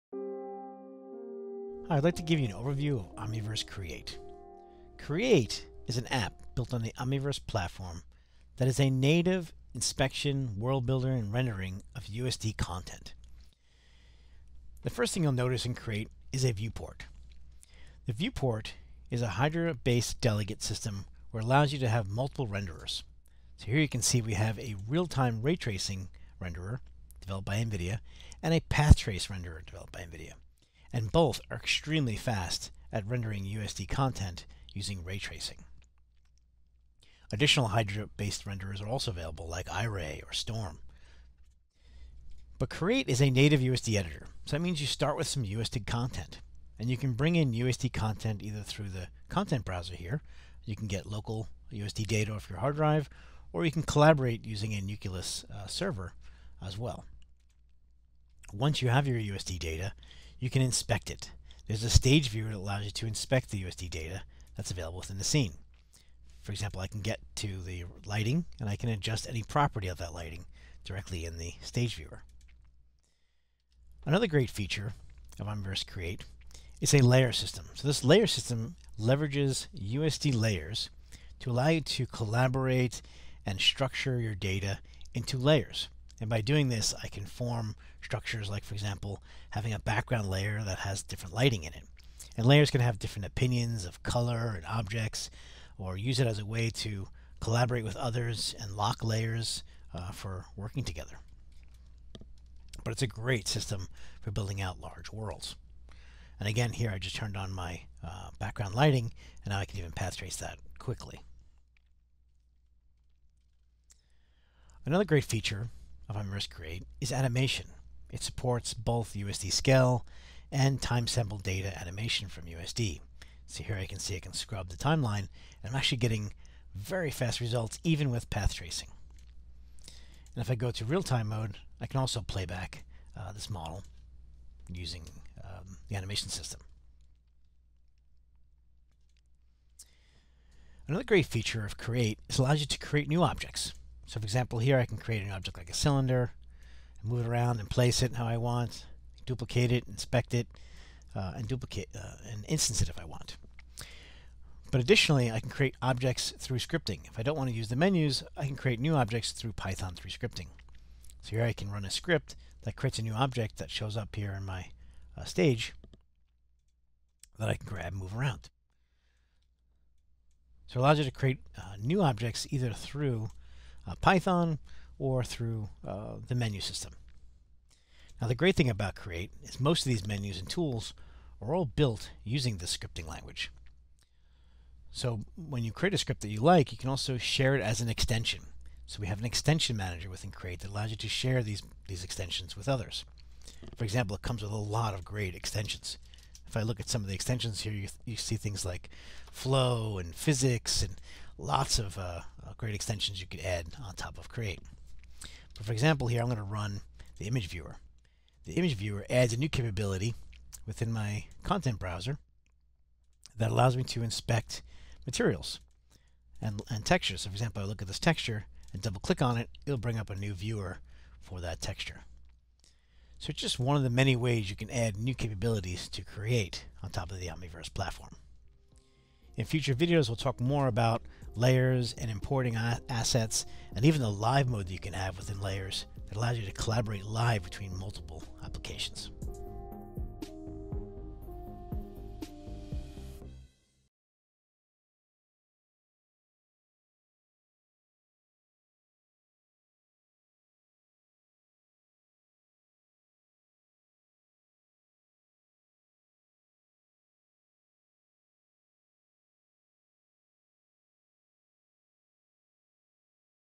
Omniverse Create Overview | NVIDIA Omniverse Tutorials
Omniverse Create accelerates advanced scene composition and allows users to assemble, light, simulate, and render scenes interactively in Pixar USD in real-time. Learn about the features of Create in this introductory tutorial.